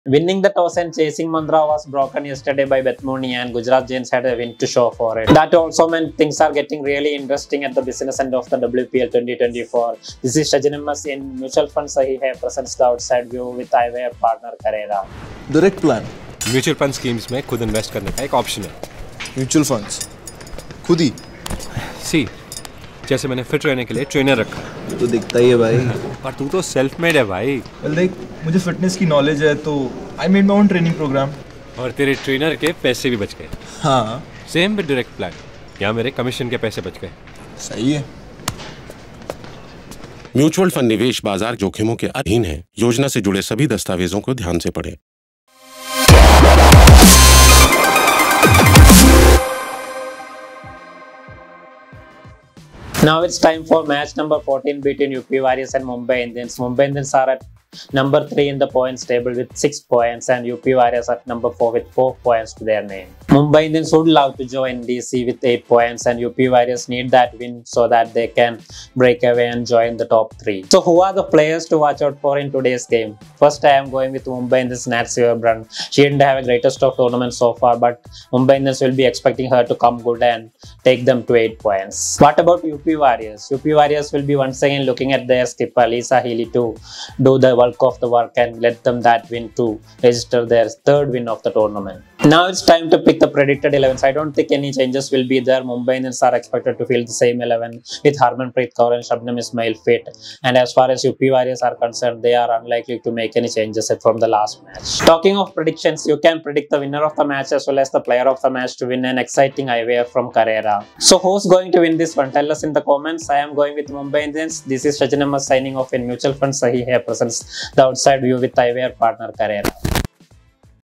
UP Warrioz vs Mumbai Indians Preview | WPL 2024 Match 14
🚨 UPWvMI Preview: Two teams looking to get back to winning ways!

Predict the winner and tonight's POTM, and you might win @carrera  eyewear! 😎